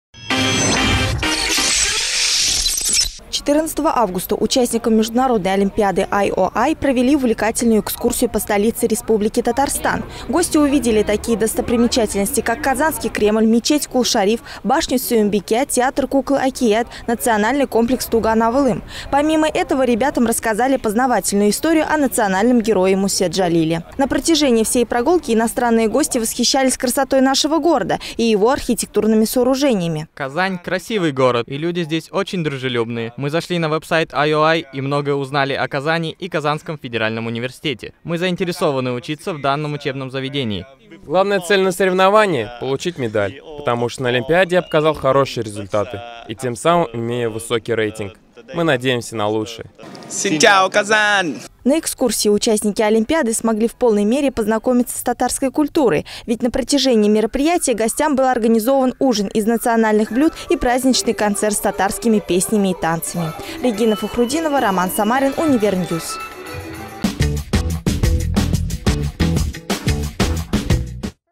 Экскурсия участников IOI от 15.08.2016
Увлекательную экскурсию по столице Республике Татарстан провели участникам Международной олимпиады IOI